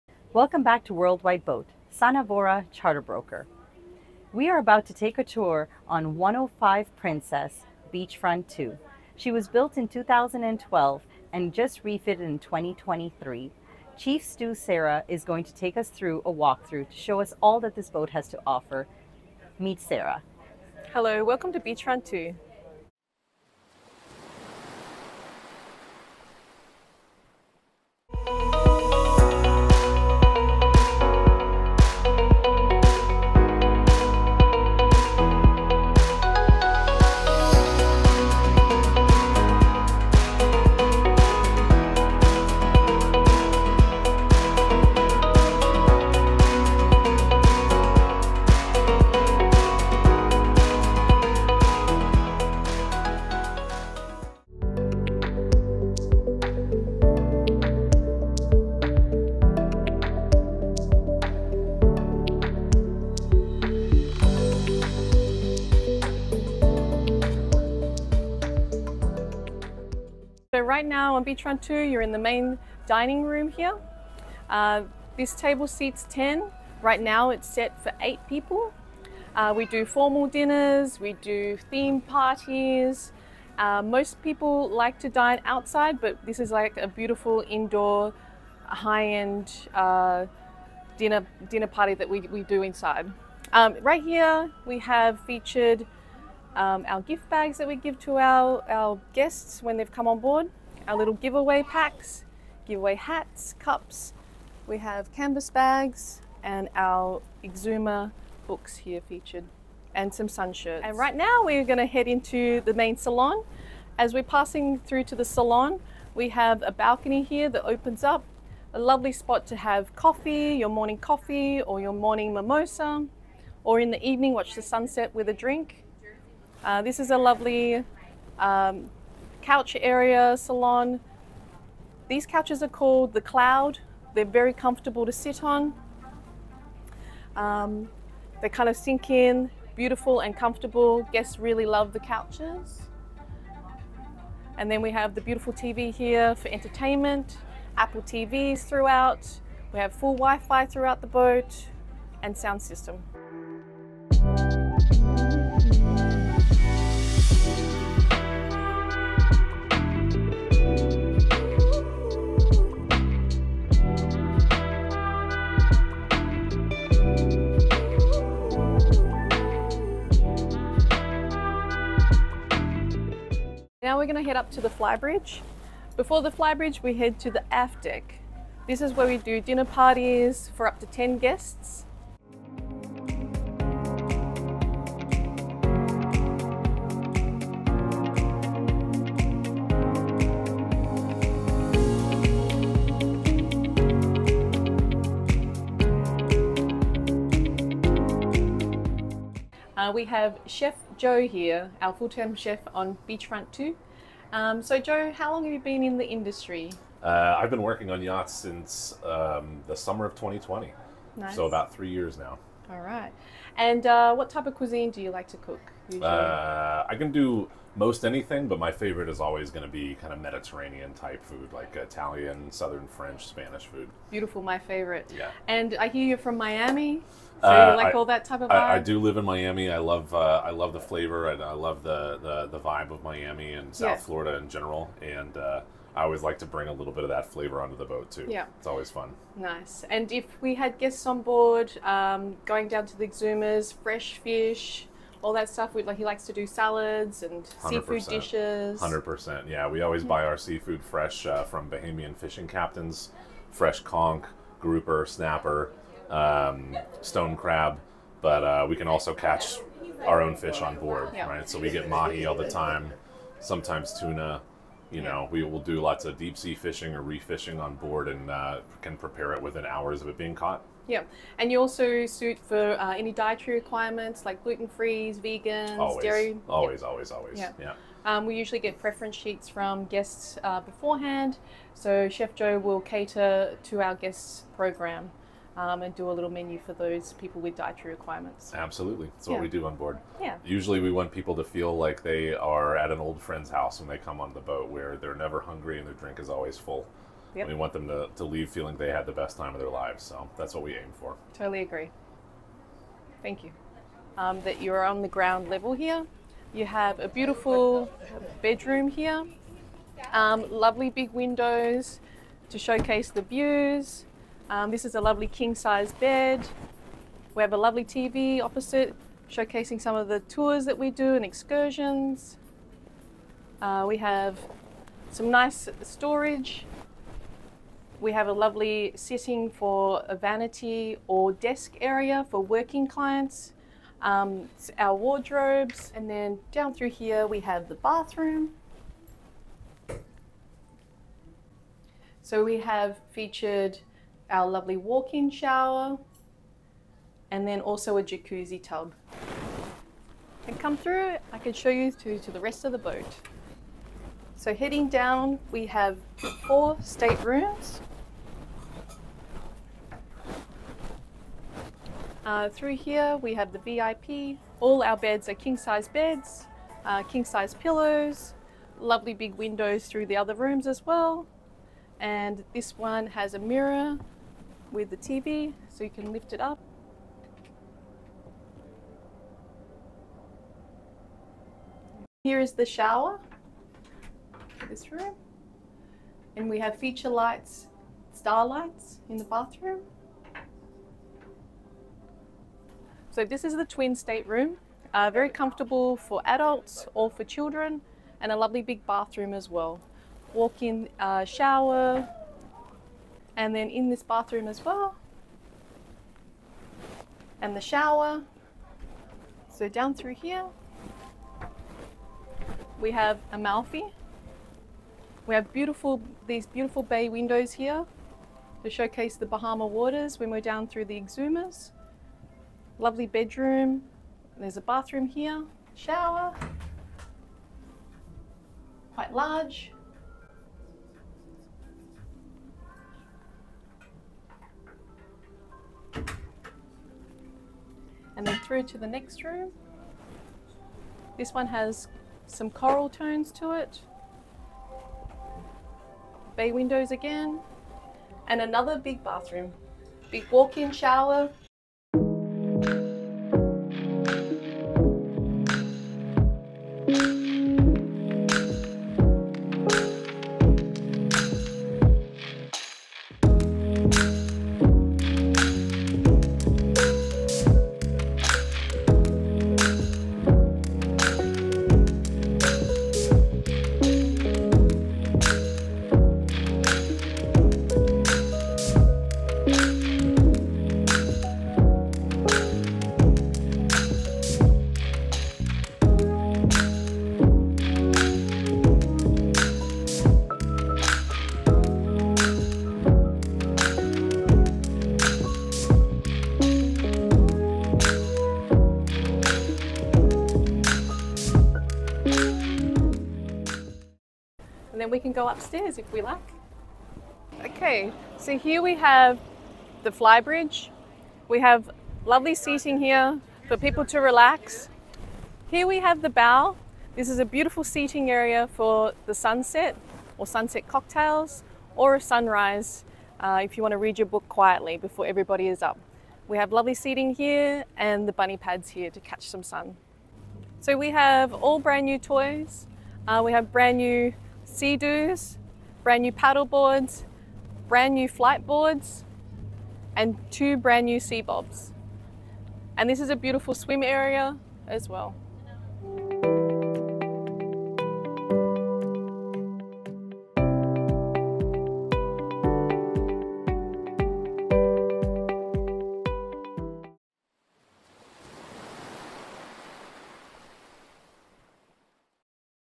What a $85,000 / week Yacht Charter in the Bahamas Looks Like!? 105’ /32m Motor Yacht BEACHFRONT II
105’ / 32m Motor Yacht BEACHFRONT II (ex. Best) was built by Princess in the United Kingdom at their Plymouth shipyard. Her interior is styled by British designer design house Princess and she was completed in 2012. This luxury vessel's exterior design is the work of Bernard Olesinski and she was last refitted in 2019.

Beachfront II has been designed to comfortably accommodate up to 10 guests in 5 suites. She is also capable of carrying up to 5 crew onboard to ensure a relaxed luxury yacht experience.

Toy List:

- 2020 32ft Intrepid tender fully rigged for fishing
- Fishing gear (Trolling for big game fish and reef fishing, Hawaiian slings, Fly fishing)
- 2023 Seadoo Jetskis with audio package x 2
- 2023 Seabobs x 2
- 2023 Flite E-Foils x 2
- Paddleboards x 2
- Water skis
- Wakeboard
- Snorkeling gear
- Beach set up

Available for charter from $85,000 per week + expenses